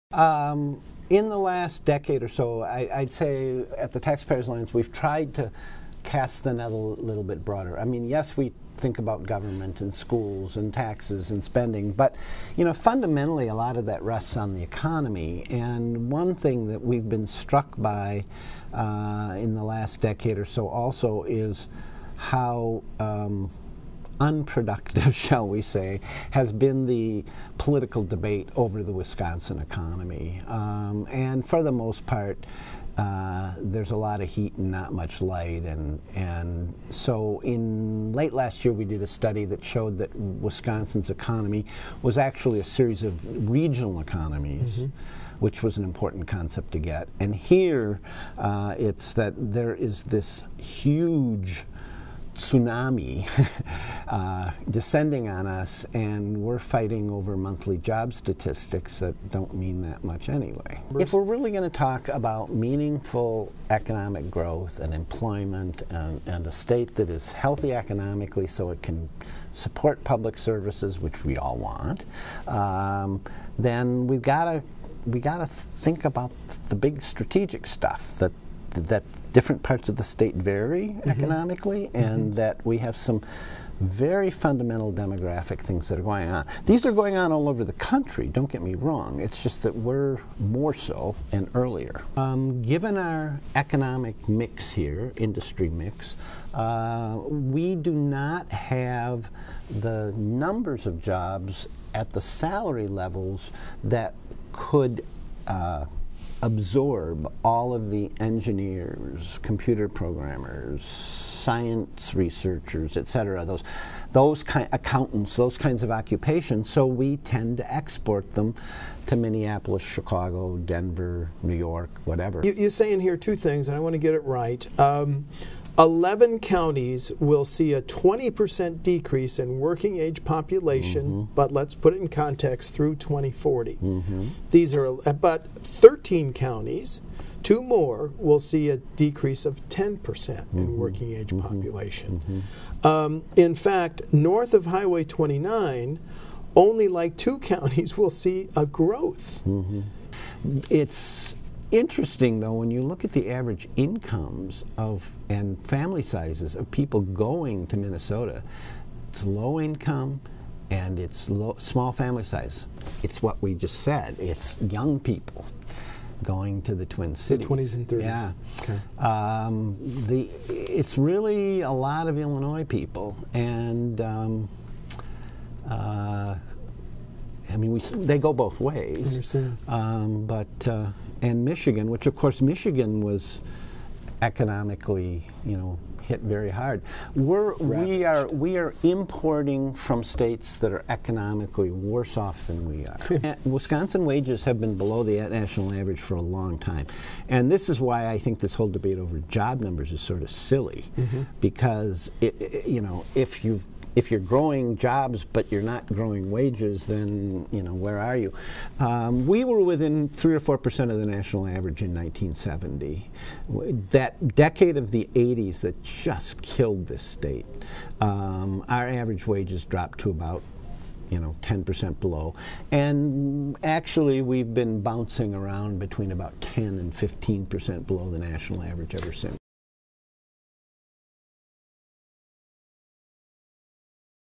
News Today: Aging Wisconsin = Fewer Workers
Dr. Todd Berry of the Wisconsin Taxpayers Alliance broke down a new report that details Wisconsin's impending shortage of qualified young workers.